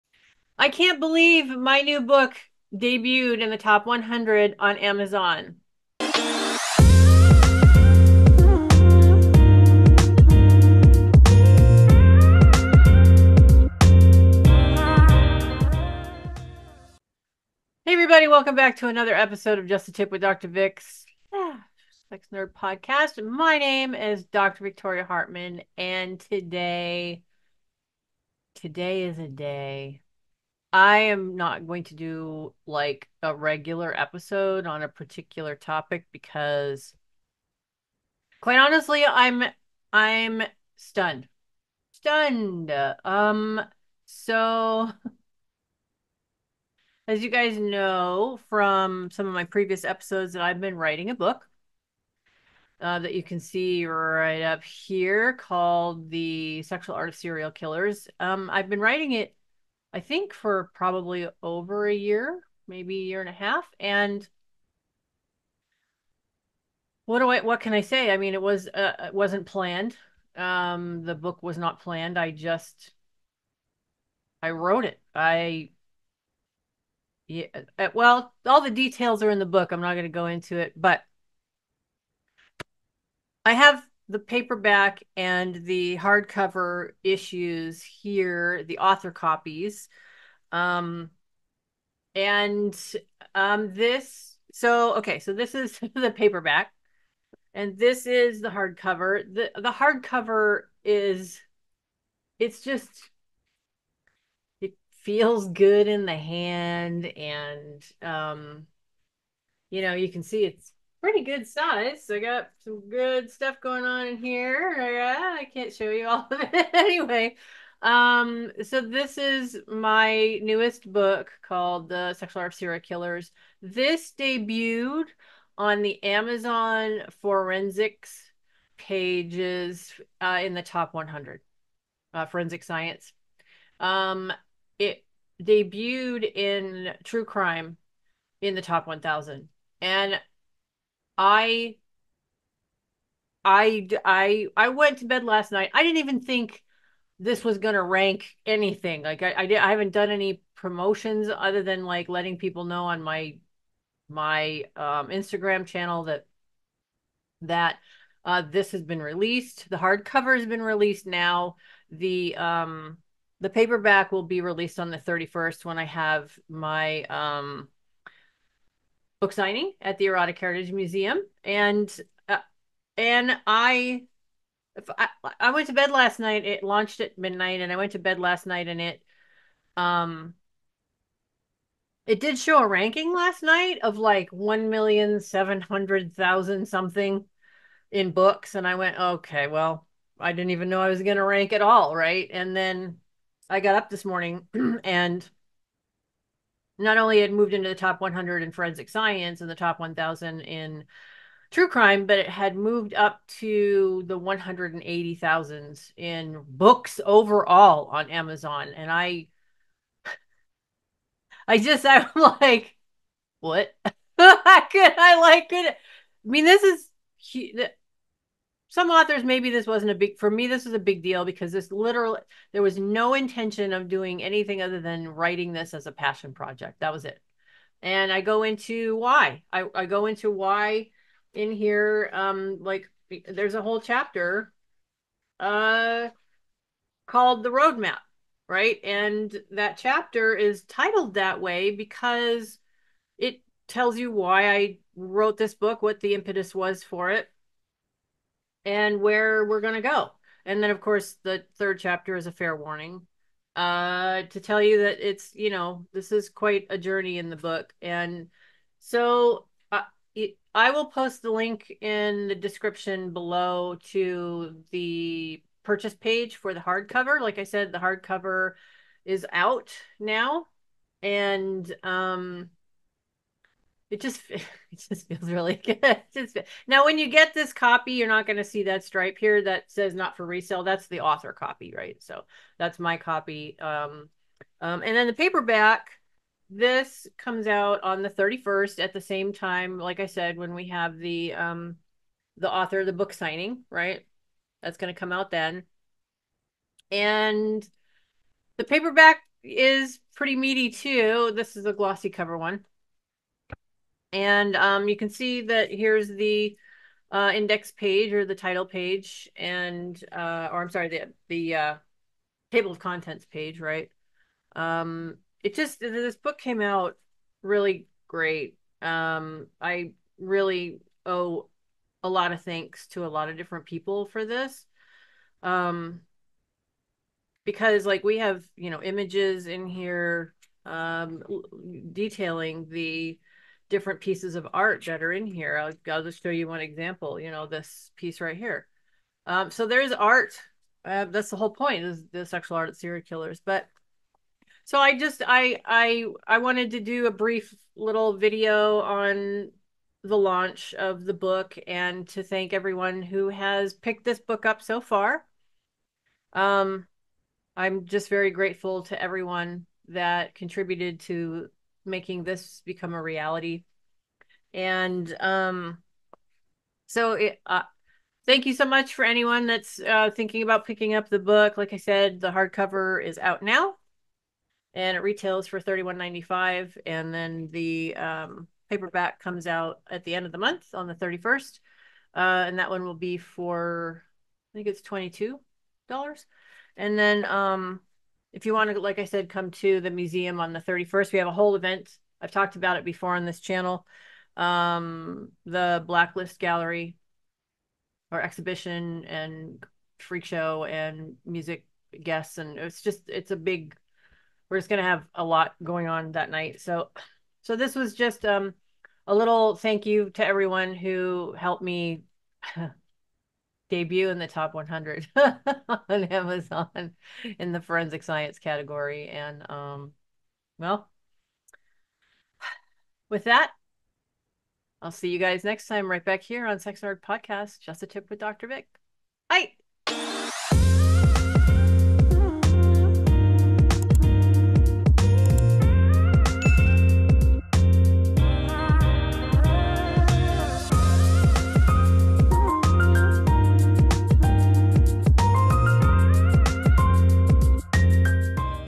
AMAZON TOP 100 DEBUT!!!
I am beside myself with joy as the book debut in the Top 100 in Forensic Science among other metrics!
Check out the episode for more info!

MY NEWEST BOOK IS NOW LIVE AND AVAILABLE FOR PURCHASE!